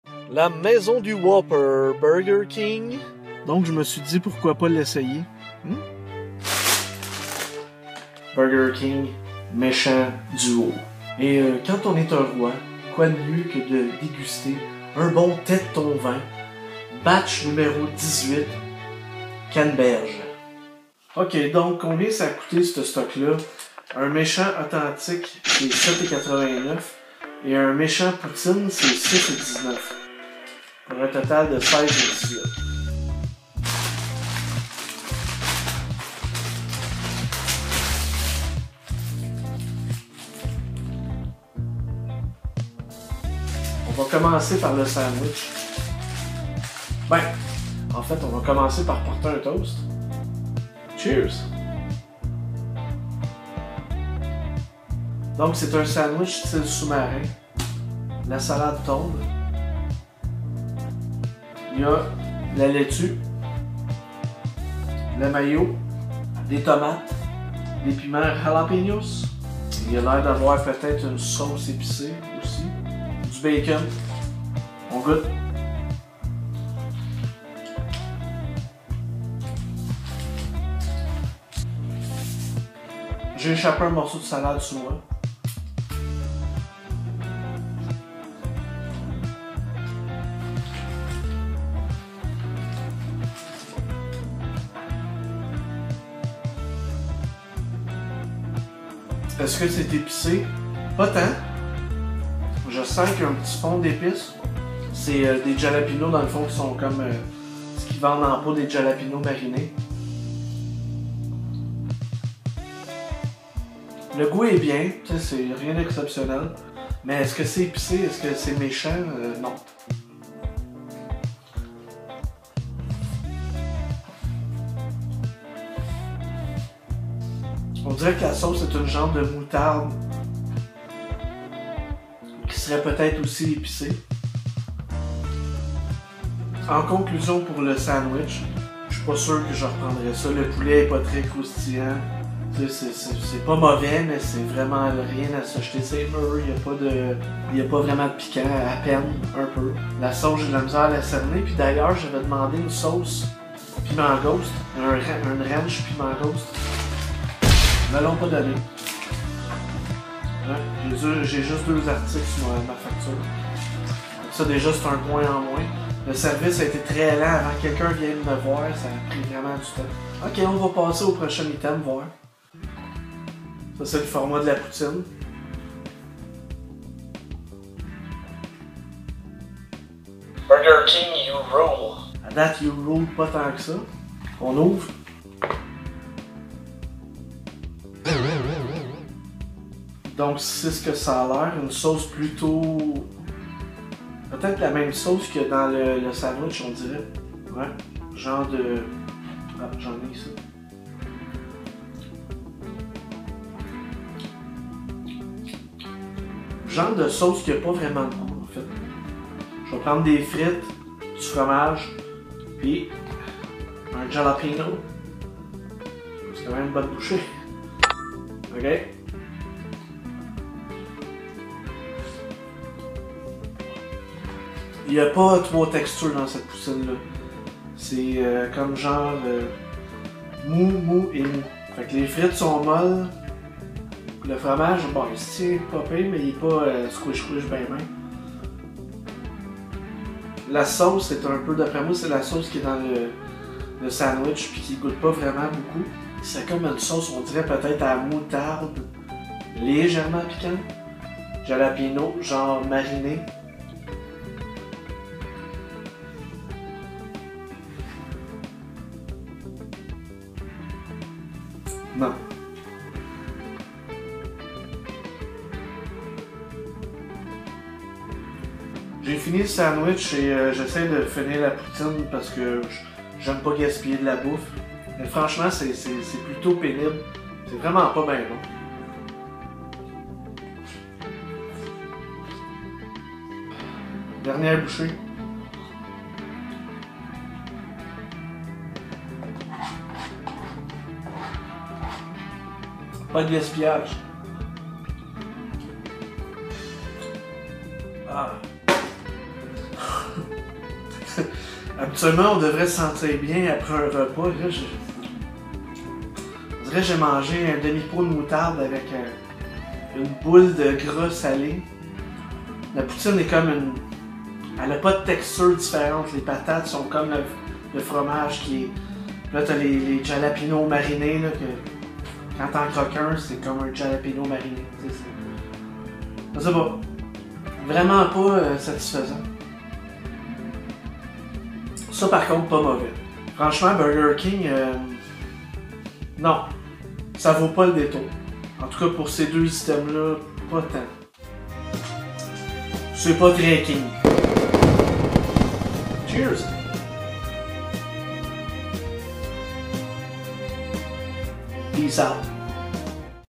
Burger King
Dégustation du méchant sandwich au poulet & de la méchante poutine
plus la 18ème batch du Tètes Ton Vin inspiré de la recette de Pawpaw
de la chaîne @DoingItCheap
par TTVfilms 2024